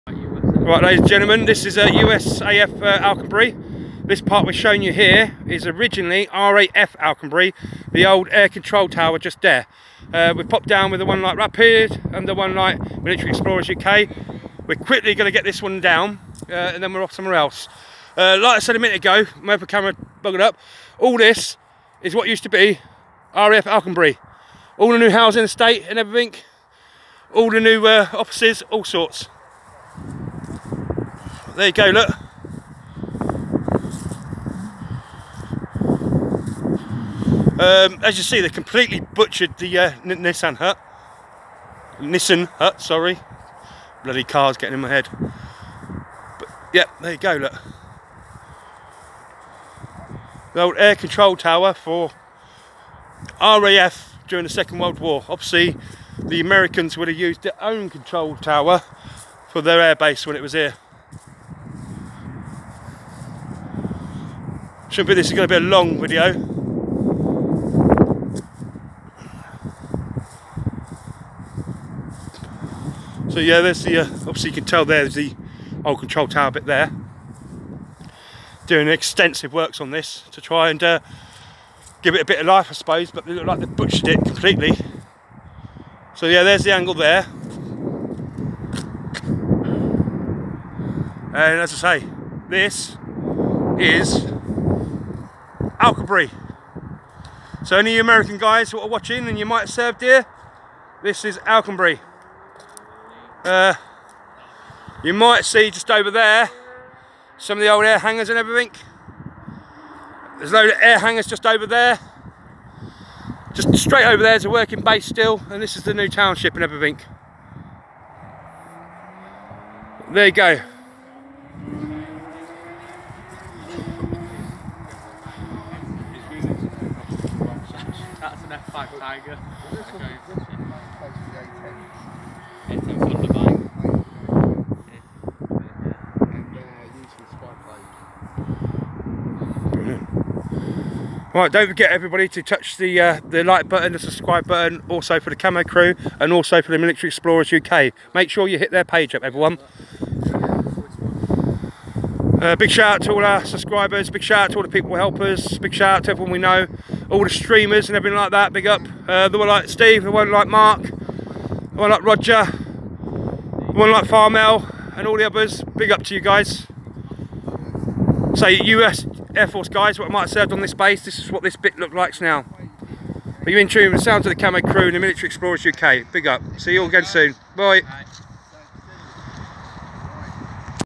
Raf Alconbury WW2 Watch Tower Renovation
We went along over year ago now to see this as it was being done and knew we would have to come back and see the out come now . One good thing to come out of lock down ,bit more time to exercise ,we cover the finished project in the next video .